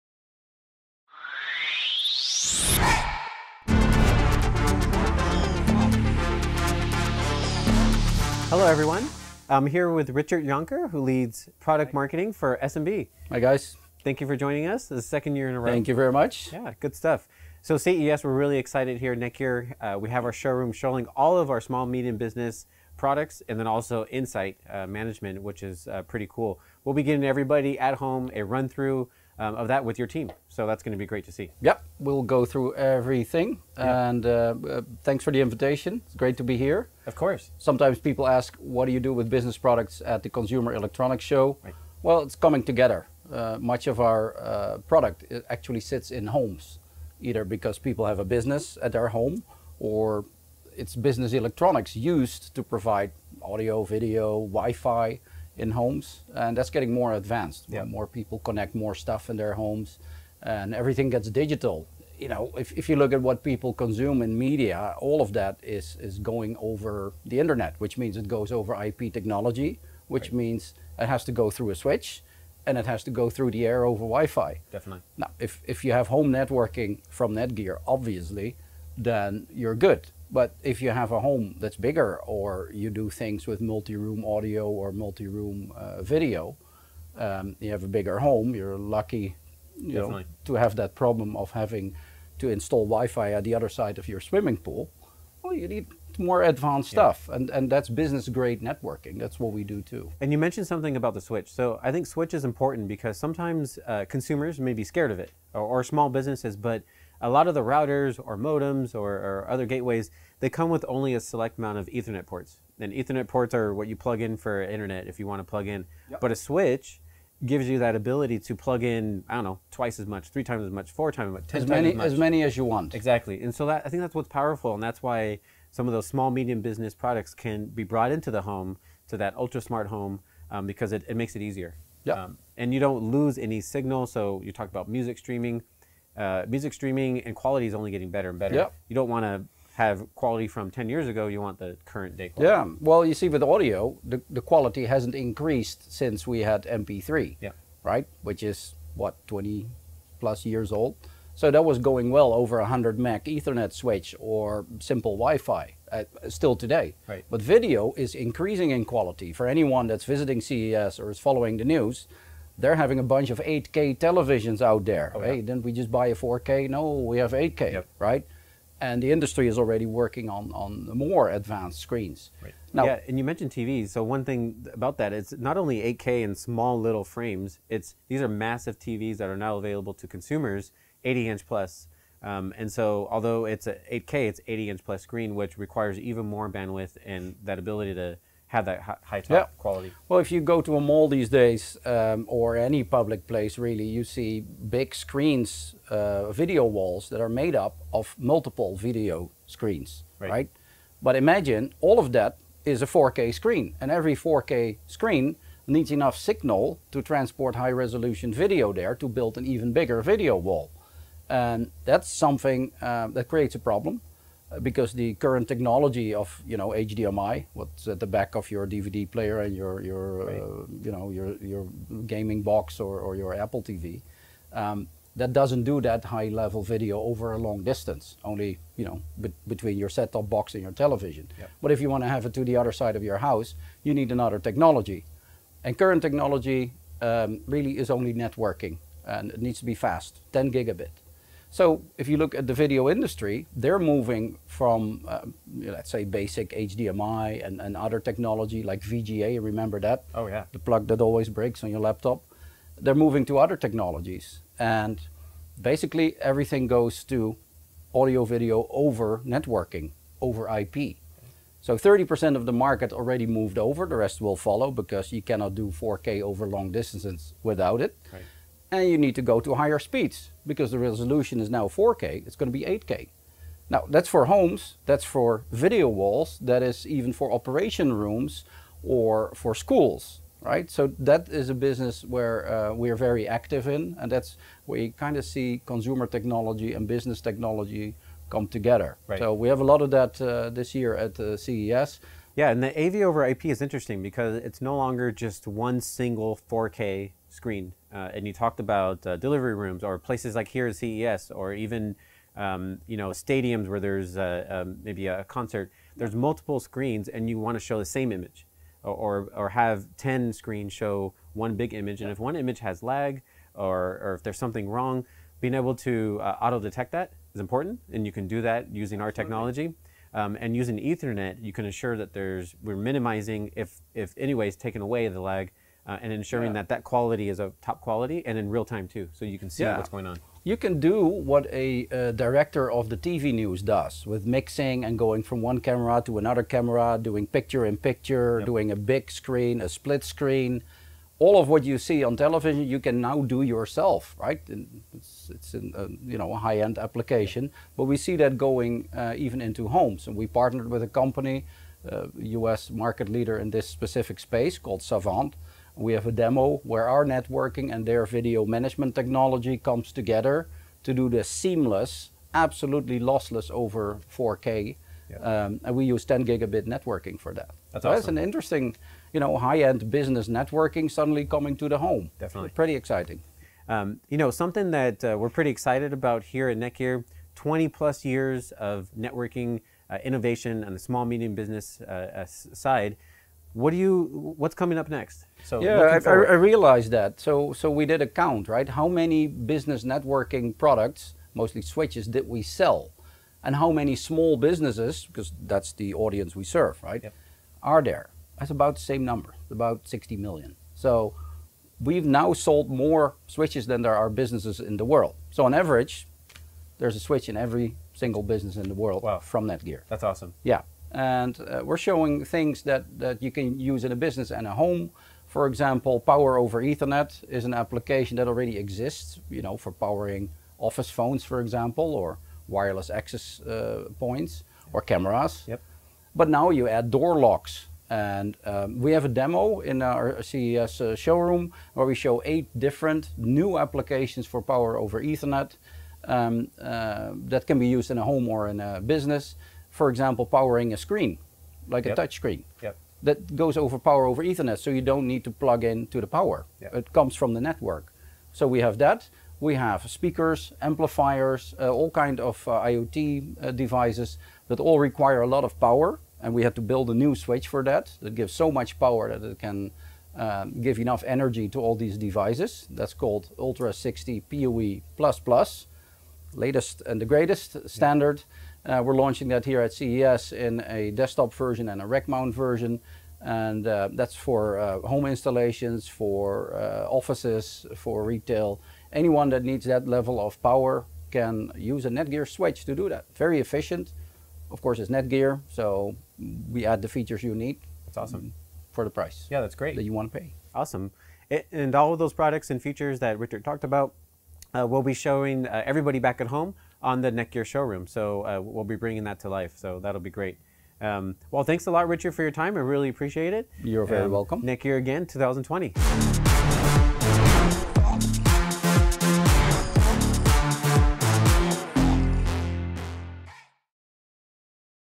Bridging the Gap Between Home & Business: Interview with Richard Jonker | NETGEAR
What is NETGEAR Business doing at the world's biggest tech tradeshow for consumers? Hear it from Richard Jonker himself, VP of SMB Product Management at NETGEAR. Get business-grade solutions that's affordable not just for your enterprise, but your home office as well.

Richard Jonker leads NETGEAR Business, powering the networks of small and medium-sized businesses around the world. In this interview, Richard and Chris talk about how NETGEAR is bridging the gap between commercial and home solutions, the various applications of NETGEAR Business switches, and how this will disrupt different industries in the upcoming years.

Richard Jonker, VP of SMB Product Management, leads the teams who build the switches, access points, and other solutions that make it easy to configure and manage networks for small and medium-sized businesses. Get helpful insights from the leaders of NETGEAR, as they talk industry, innovation, and what’s next in the world of SMB networking.